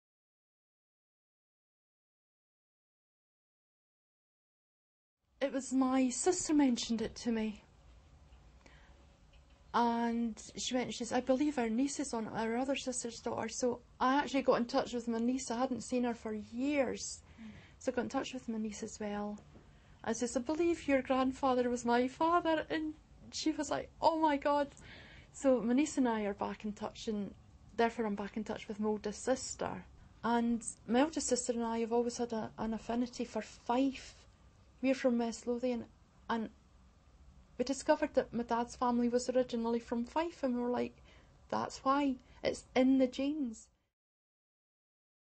Sheila's Story
This video was filmed and prepared by Genes Reunited. We loved Sheila's story so much we wanted to share it with all of our members.